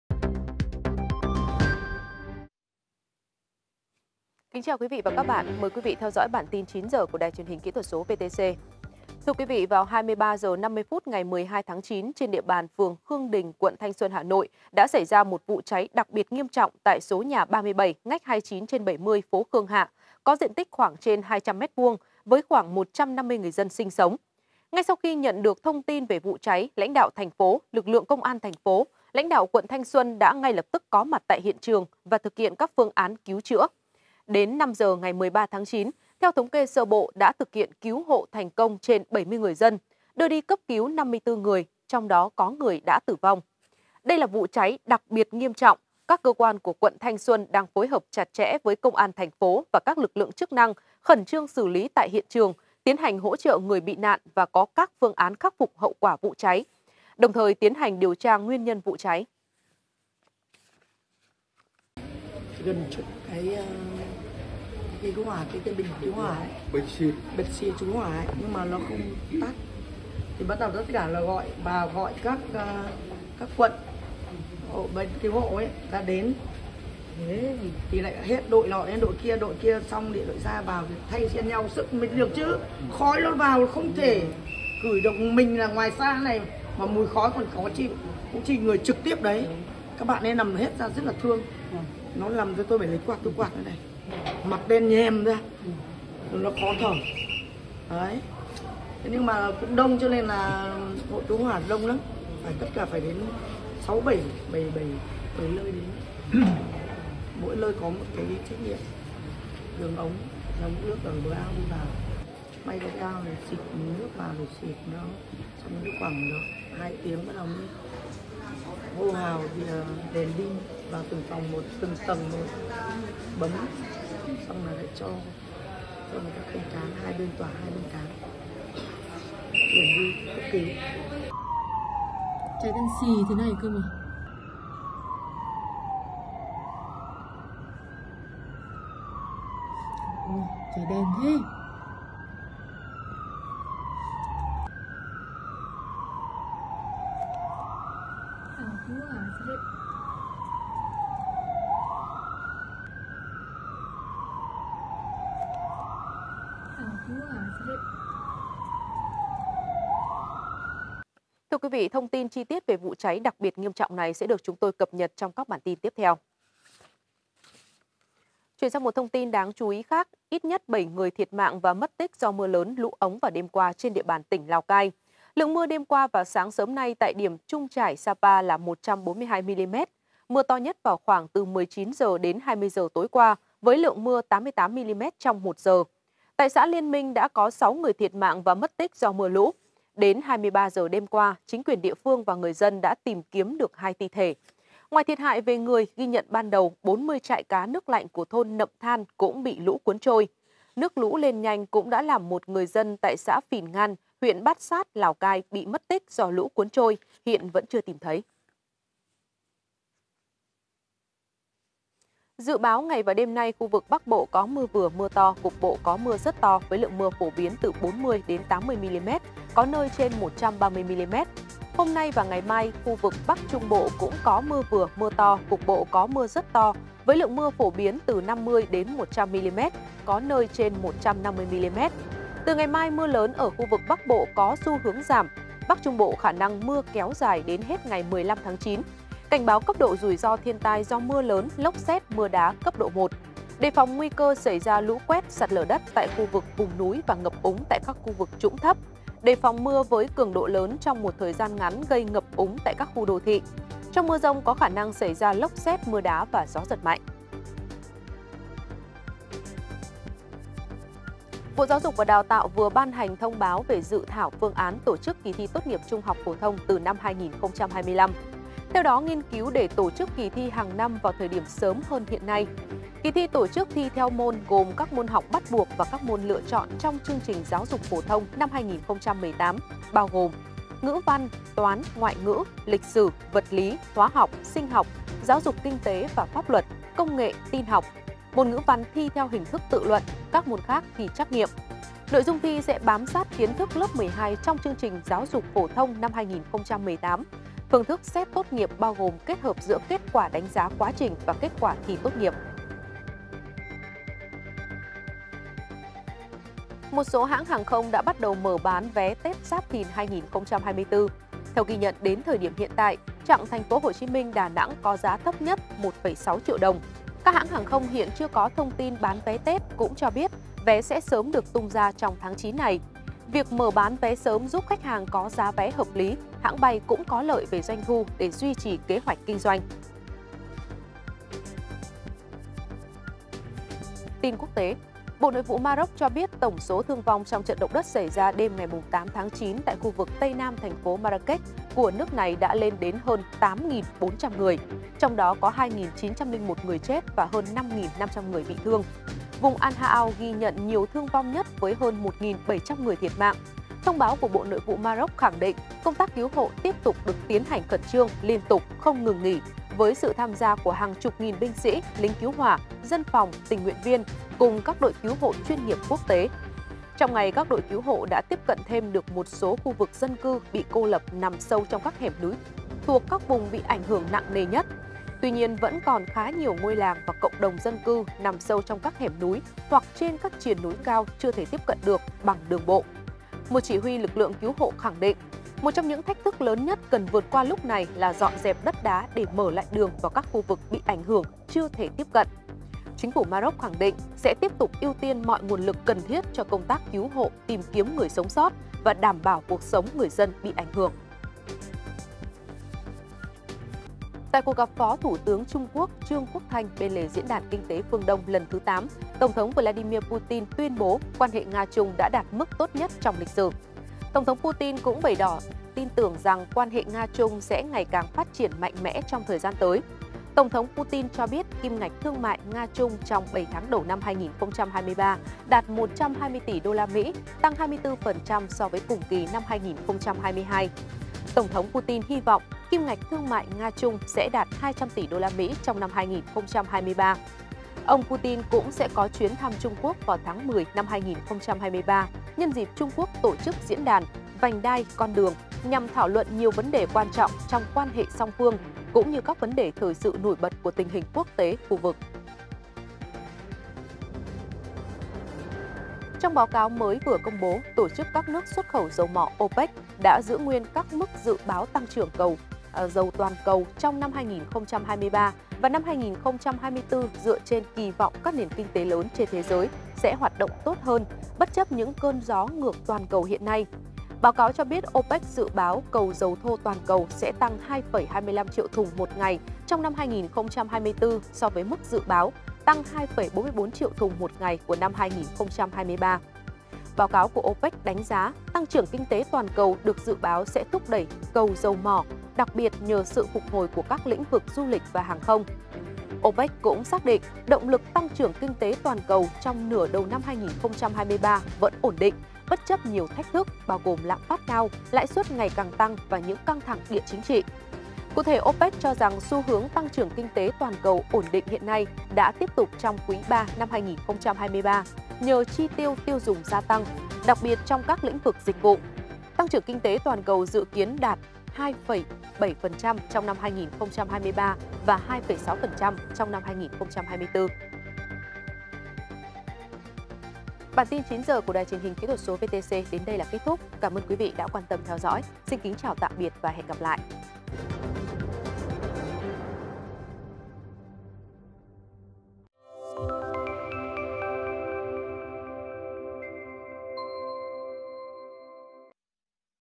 Cháy chung cư mini ở Hà Nội: Cứu được hơn 70 người, đưa đi cấp cứu 54 người | VTC16
VTC16 | Theo tin từ UBND TP Hà Nội, liên quan đến vụ cháy chung cư mini tại quận Thanh Xuân, thống kê sơ bộ, lực lượng chức năng đã cứu hộ được 70 người dân, đưa đi cấp cứu 54 người, trong đó có người đã tử vong.